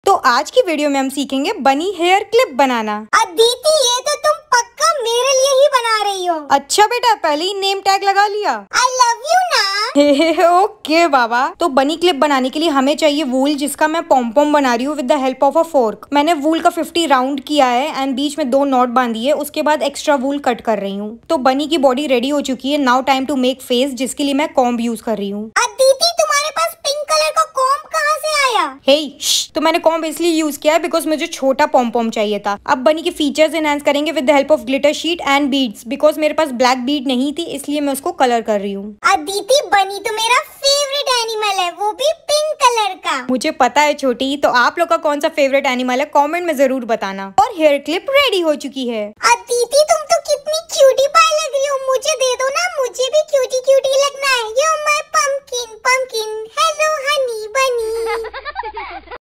DIY Bunny Hairclip 😱
I'm back with fun DIY. This Bunny Hair Clip is so easy to make and I personally enjoyed the process. Do recreate and share your creation with me on my Instagram Page and get featured.